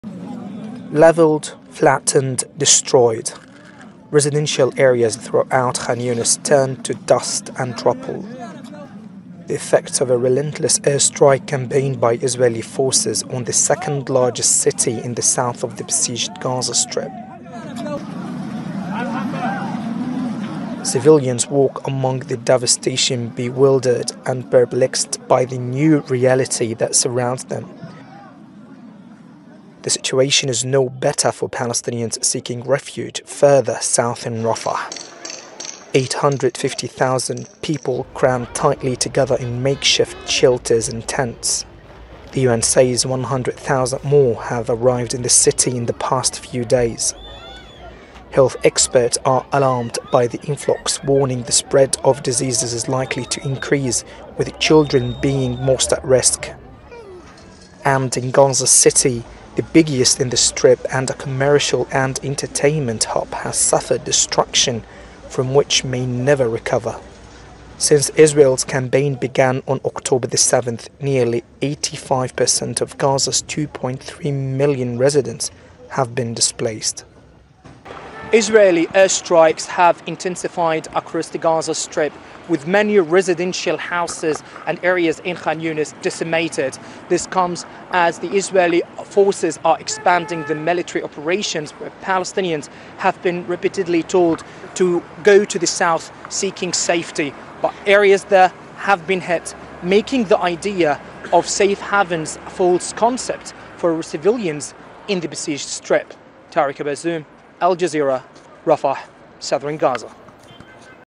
Southern Gaza operation Israel expanding operations around Khan Younis
21,672 Palestinians have now been killed in Gaza since the war began. Israeli warplanes carried out intense airstrikes on Khan Younis. Israel's military says it is expanding operations there to target Hamas fighters. But many civilians have been killed.

ِAl Jazeera's Tareq Abu Azzoum reports.